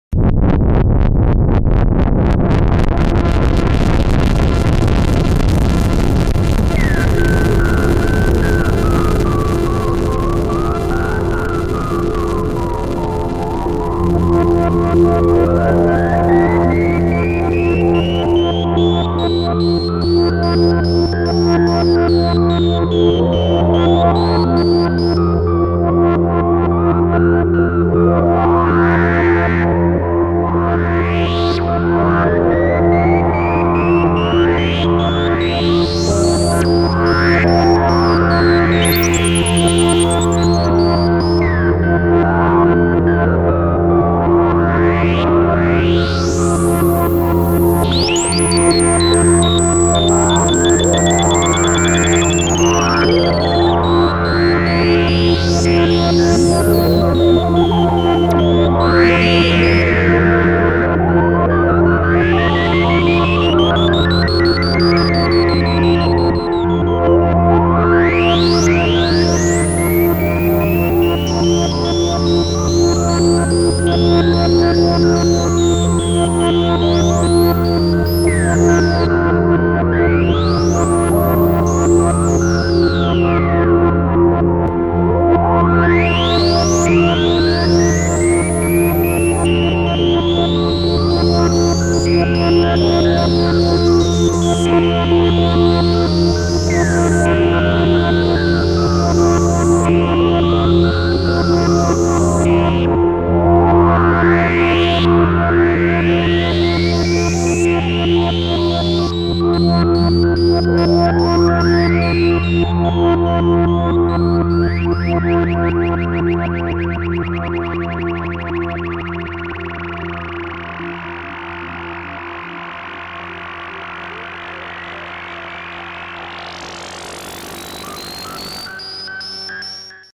Steelphon S900 demo... one of the best analog synthesizer made in Italy
Steelphon S900... 2 oscillators analog monophonic synthesizer Made in Italy. Multitrack recording in 2007.